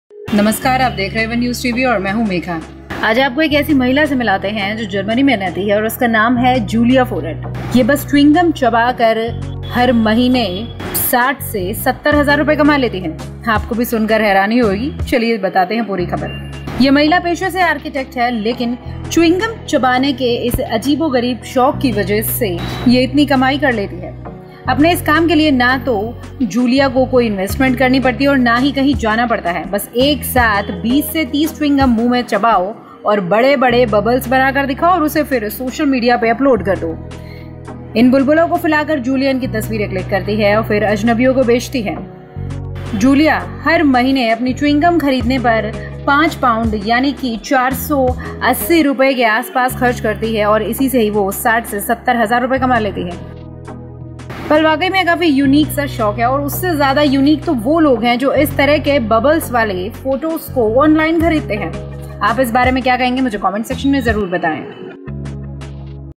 सिर्फ मुहँ फुलाकर ये महिला कमाती है 70 हजार रुपए महीना | julia forat chewing gum balloon:viral video
ये महिला च्युइंग गम चबाकर कमा रही है 70 हजार रुपए | julia forat chewing gum balloon
Aonenewstv